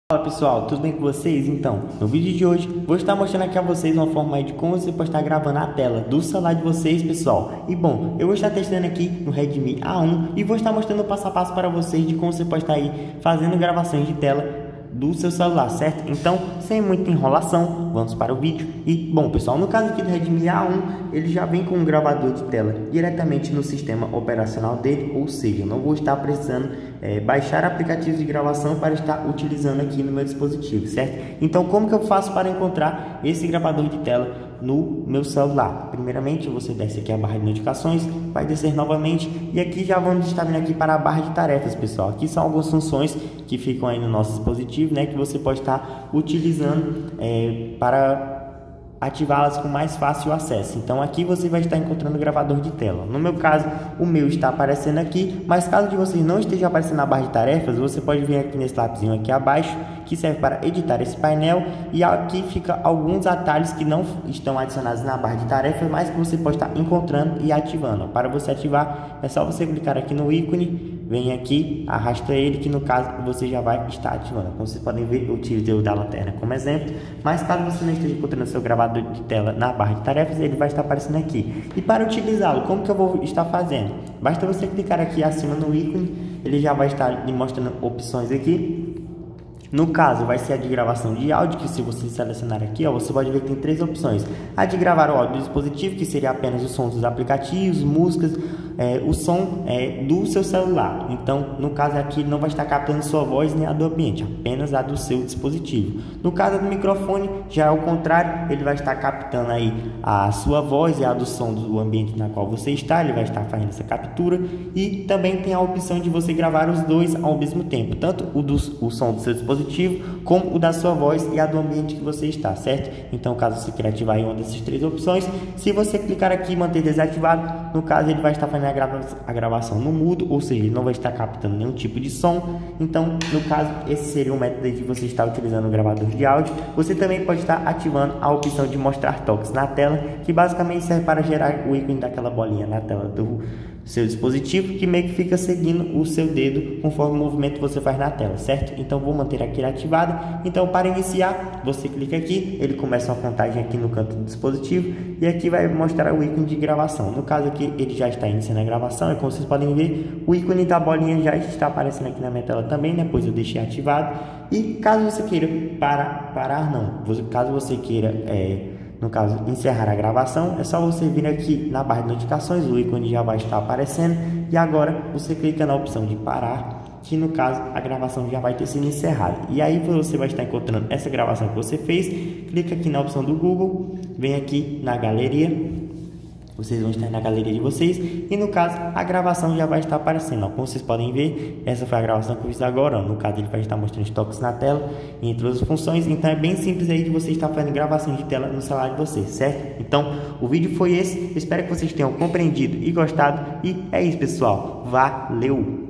Como gravar a tela do Redmi A1 - Passo a passo
Fala pessoal!! Nesse video estarei mostrando uma forma de Como gravar a tela do Redmi A1.